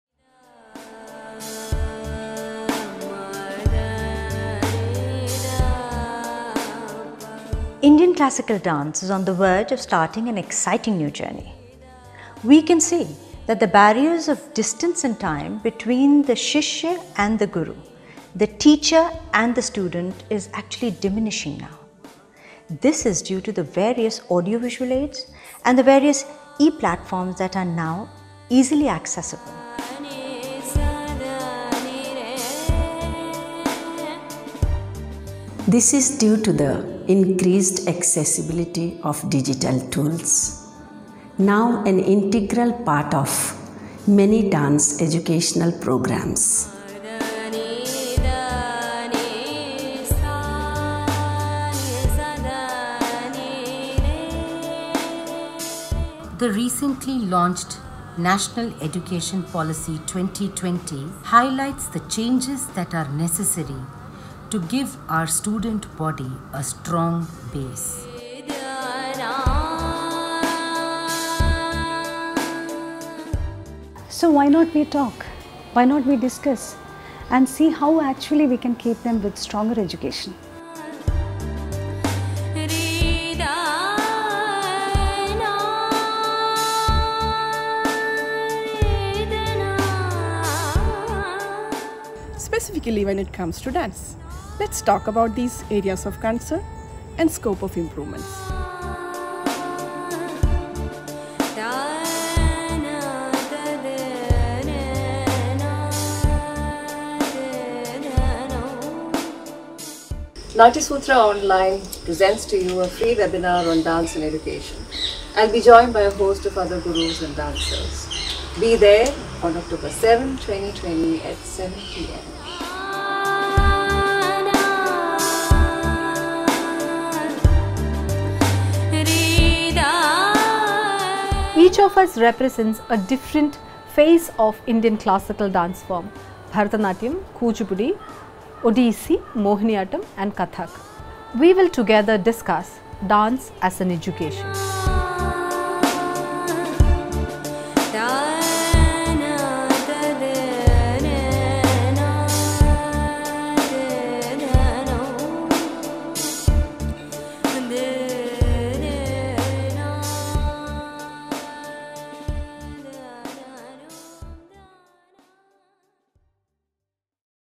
Panel Discussion on Dance as an Education | 7 October 2020 | Webinar Moderated by Guru Pali Chandra
Date: 7th October 2020, Time: 7 PM IST, Duration: 90 minutes

Discourse is the most elegant way to reinvigorate an art form, and we invite all lovers of dance for an intimate conversation on 7th October 2020. NatyaSutraOnline hosts a free webinar to attract views and generate dialogue on the state of Dance Education in Modern India, and the paradigm shift that NEP 2020 may usher in. Guru Pali Chandra, renowned Kathak dancer and scholar, will moderate the 90-minute session. Other speakers at the event will include eminent teachers and scholars such as Vidushi Sujata Mohapatra (Odissi), Rama Vaidyanathan (Bharatanatyam), Dr. Rajashree Warrier (Bharatanatyam), Smitha Rajan (Mohiniyattam), Dr. Methil Devika (Mohiniyattam) and Sreelakshmy Govardhanan (Kuchipudi).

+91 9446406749